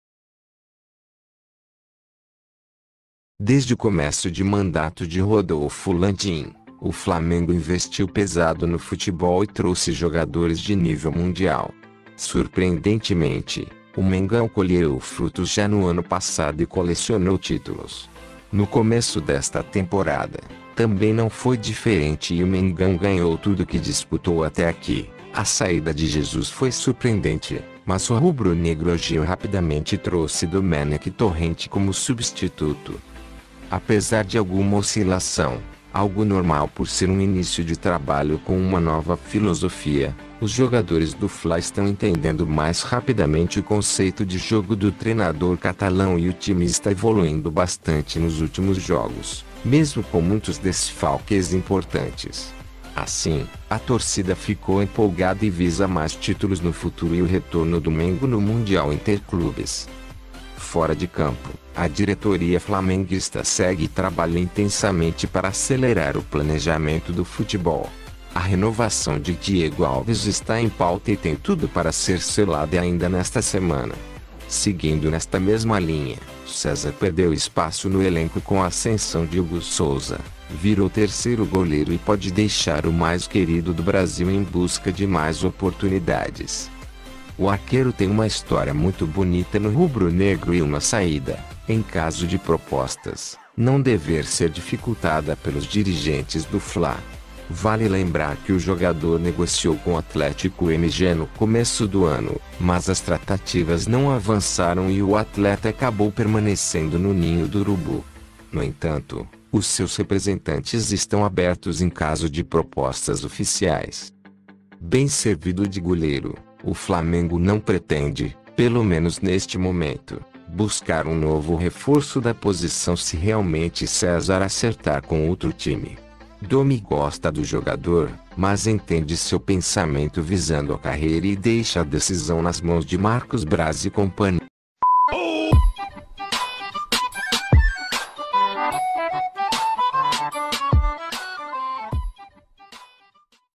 *BOMBA!!!* XODÓ DA TORCIDA ANÚNCIA SAÍDA!!!
Desde o começo de mandato de Rodolfo Landim, o Flamengo investiu pesado no futebol e trouxe jogadores de nível mundial. Surpreendentemente, o Mengão colheu frutos já no ano passado e colecionou títulos. No começo desta temporada, também não foi diferente e o Mengão ganhou tudo que disputou até aqui; a saída de Jesus foi surpreendente, mas o Rubro-Negro agiu rapidamente e trouxe Domènec Torrent como substituto.

Apesar de alguma oscilação, algo normal por ser um início de trabalho com uma nova filosofia, os jogadores do Fla estão entendendo mais rapidamente o conceito de jogo do treinador catalão e o time está evoluindo bastante nos últimos jogos, mesmo com muitos desfalques importantes. Assim, a torcida ficou empolgada e visa mais títulos no futuro e o retorno do Mengo no Mundial Interclubes.

Fora de campo, a diretoria flamenguista segue trabalho intensamente para acelerar o planejamento do futebol. A renovação de Diego Alves está em pauta e tem tudo para ser selada ainda nesta semana. Seguindo nesta mesma linha, César perdeu espaço no elenco com a ascensão de Hugo Souza, virou terceiro goleiro e pode deixar o Mais Querido do Brasil em busca de mais oportunidades.

O arqueiro tem uma história muito bonita no Rubro-Negro e uma saída, em caso de propostas, não dever ser dificultada pelos dirigentes do Fla. Vale lembrar que o jogador negociou com Atlético-MG no começo do ano, mas as tratativas não avançaram e o atleta acabou permanecendo no Ninho do Urubu. No entanto, os seus representantes estão abertos em caso de propostas oficiais.

Bem servido de goleiro, o Flamengo não pretende, pelo menos neste momento, buscar um novo reforço da posição se realmente César acertar com outro time. Dome gosta do jogador, mas entende seu pensamento visando a carreira e deixa a decisão nas mãos de Marcos Braz e companhia.